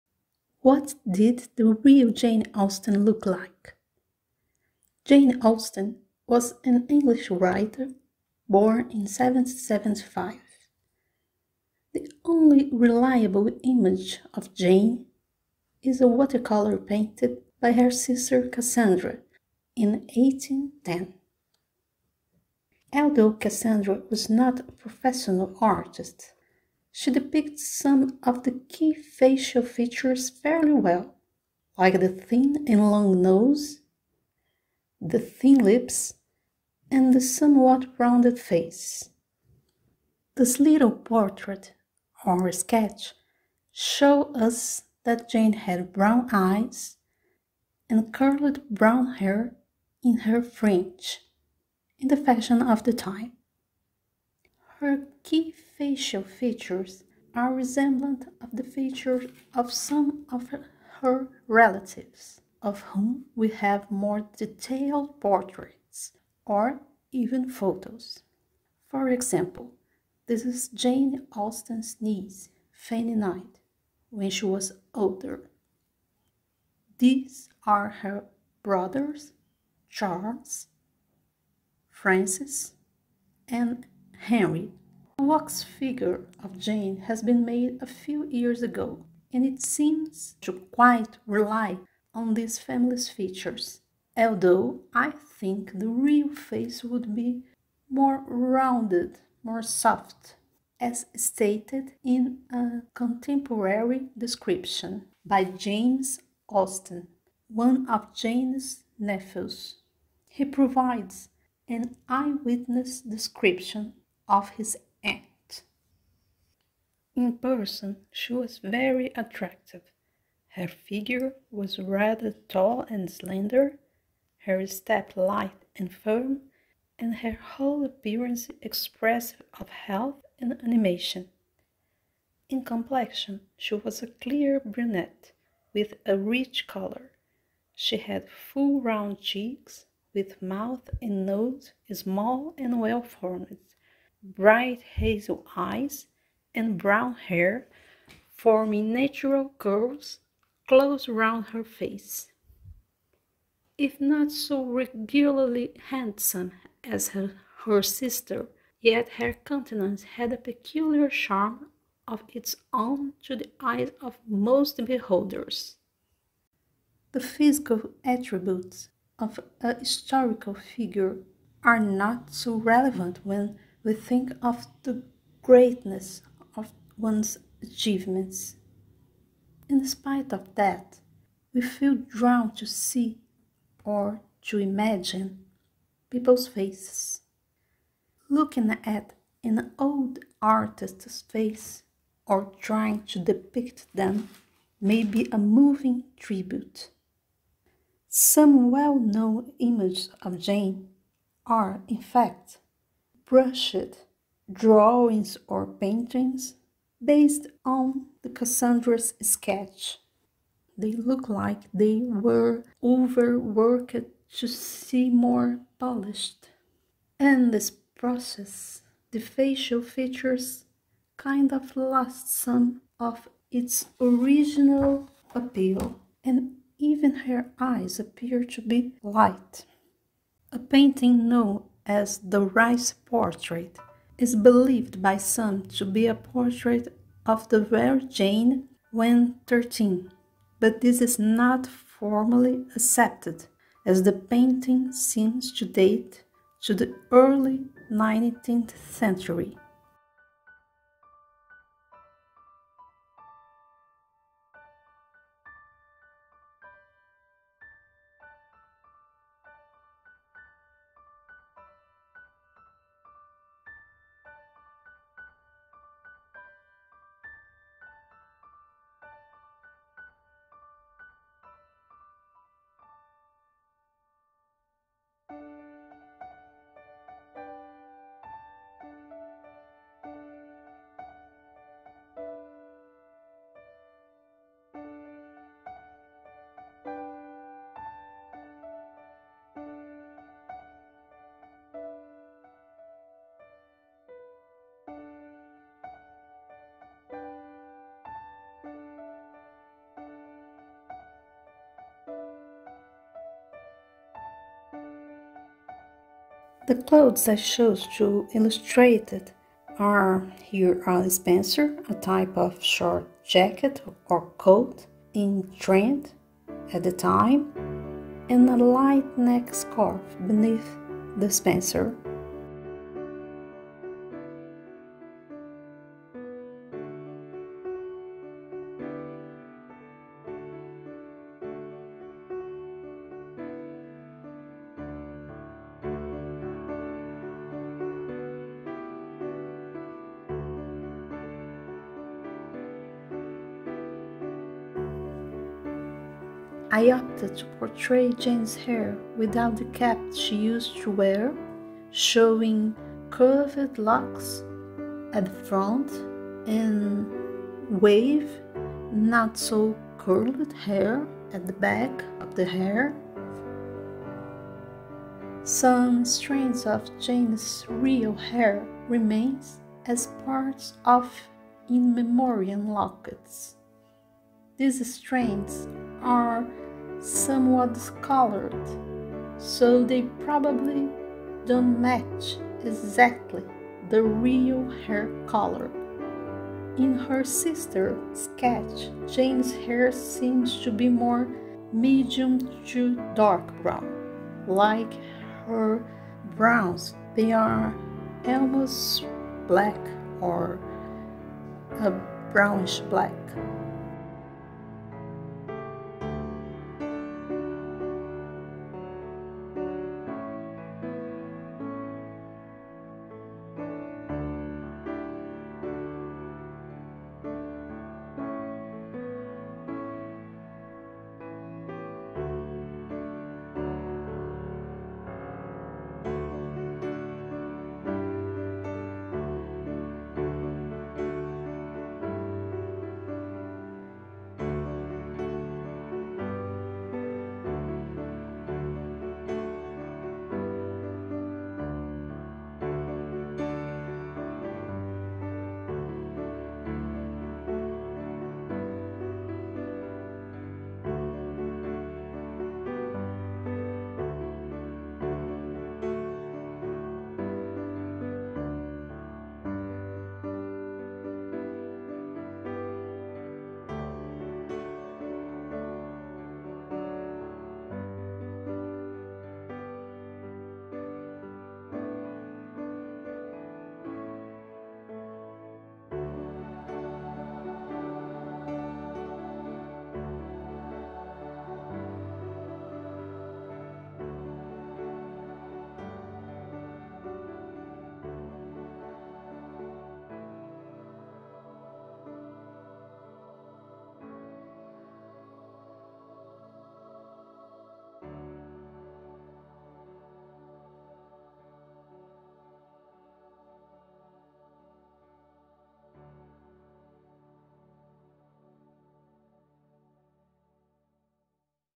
Drawing - What did Jane Austen look like?
Jane Austen face "reconstruction" by drawing, or, better saying, an artistic rendition. It's based on Cassandra's (Jane's sister) drawing and on the facial features from some family members, besides a few descriptions of her.